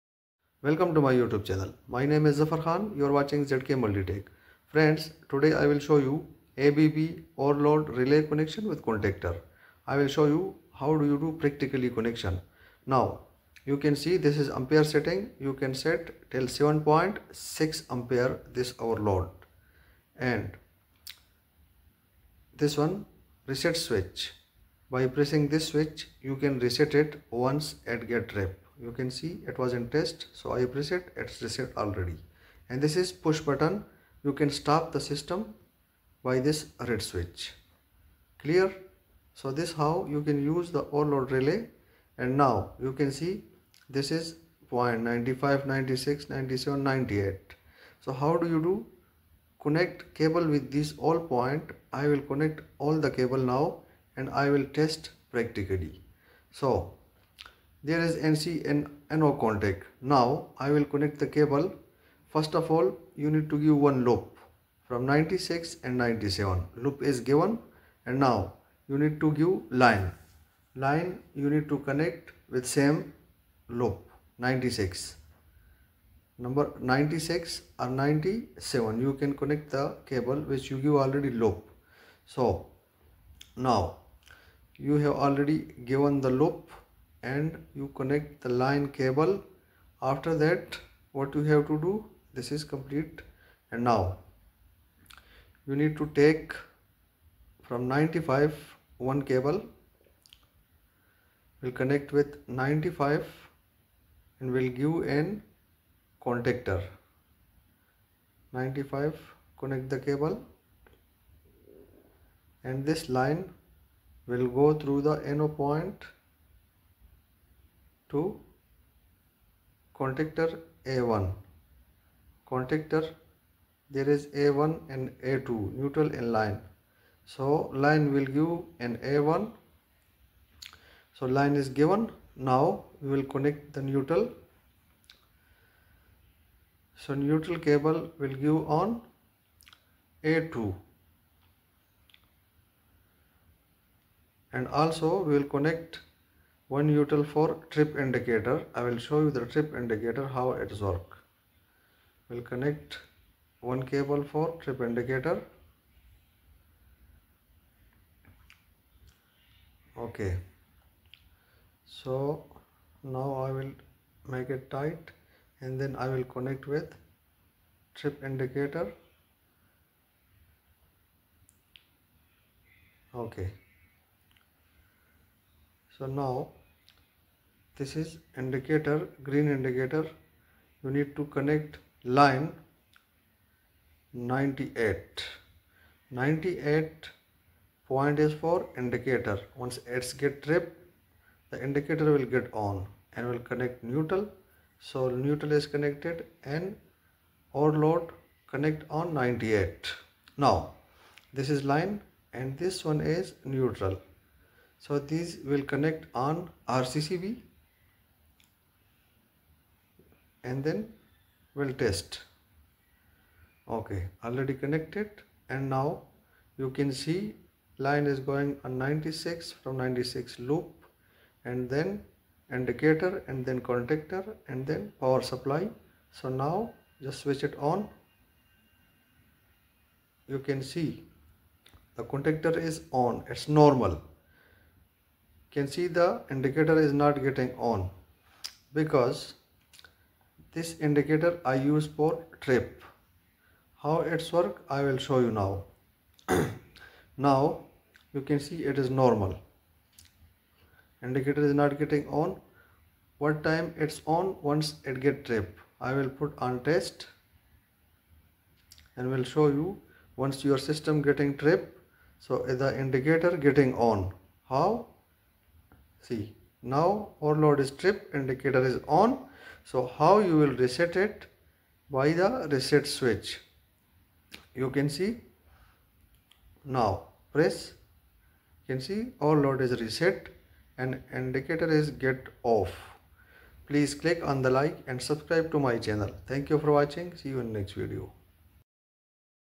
ABB overload relay connection with contactor
ABB overload relay connection practically shown in this video